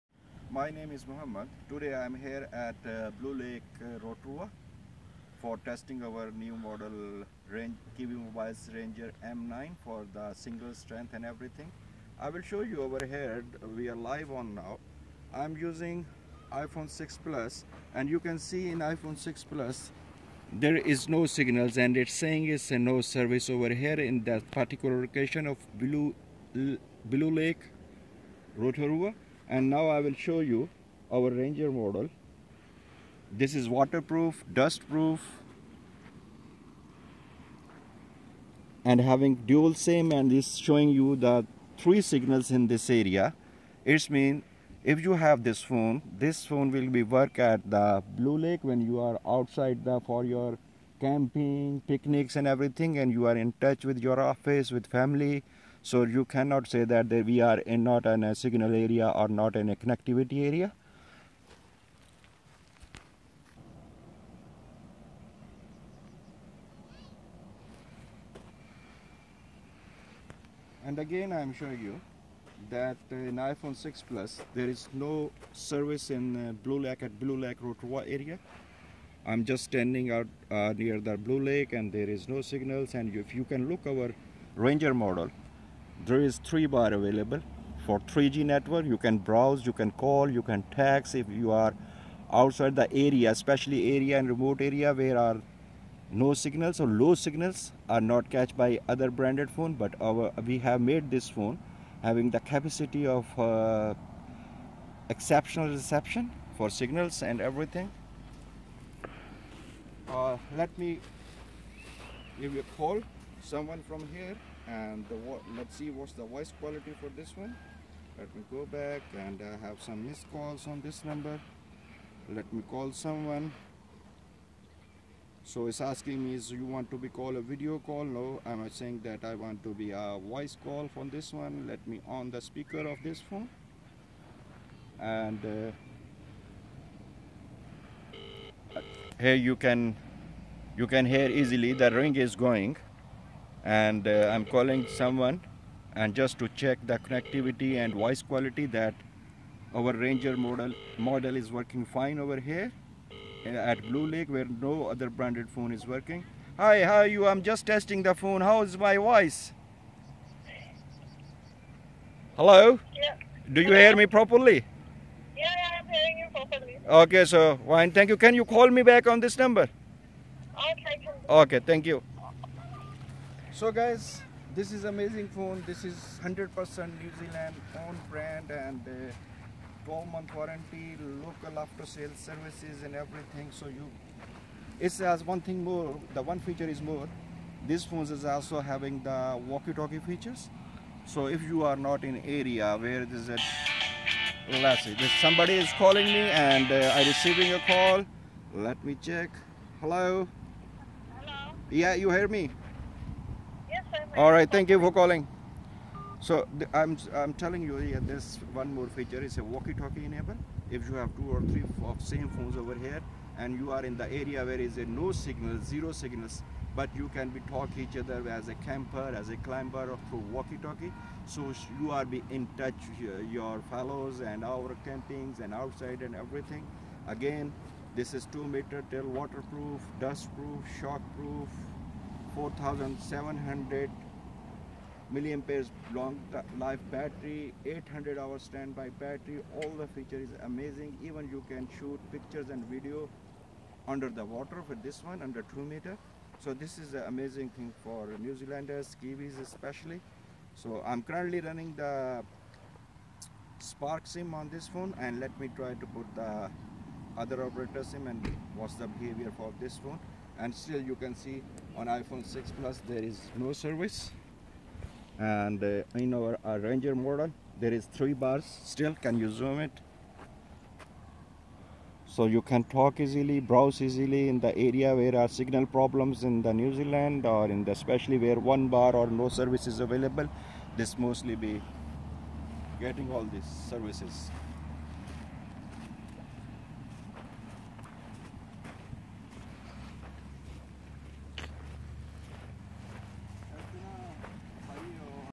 iPhone6+ Vs Kiwi Mobile Ranger M9, Signal strength test
Signal strength test drive KiwiMobile Ranger M9 Vs Iphone6+ @ Blue lake rotorua